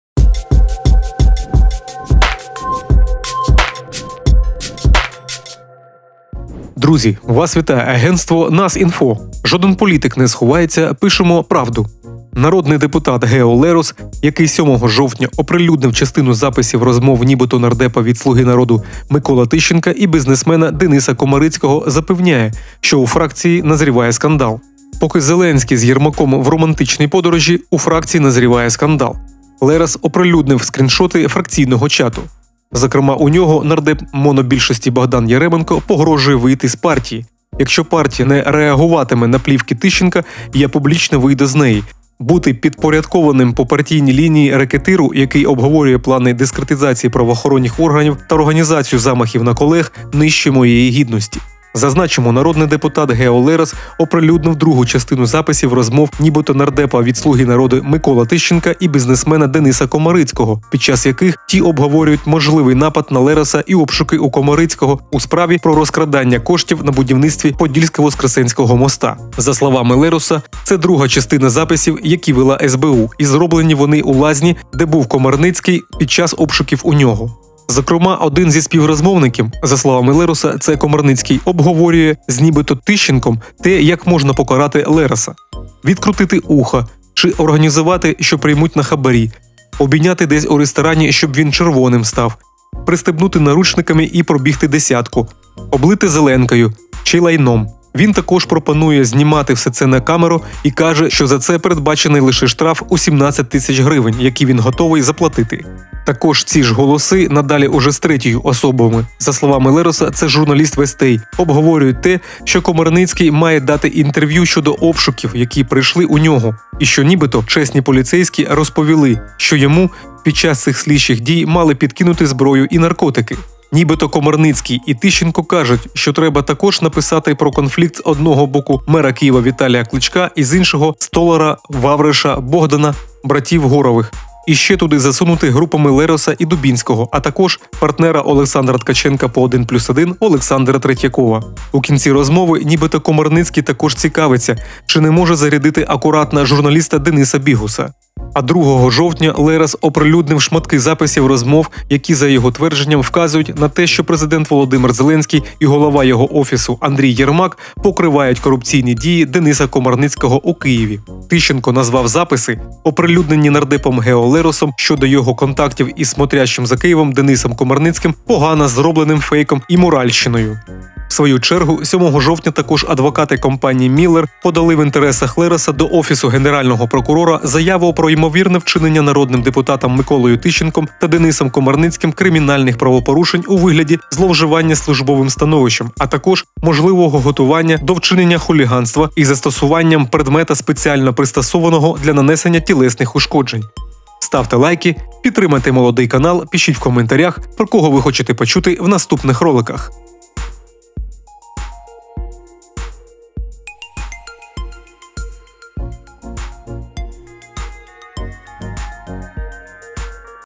ДомовинаЗЕ! Плівки лероса - забив останній цвях в кришку. Скандал в ОП
Глядачі.
Зовсім незабаром тут з'явиться сторінка в Інстаграм, Фейсбук, та посилання на наш інформаційний сайт.
А поки - насолоджуйтесь переглядом.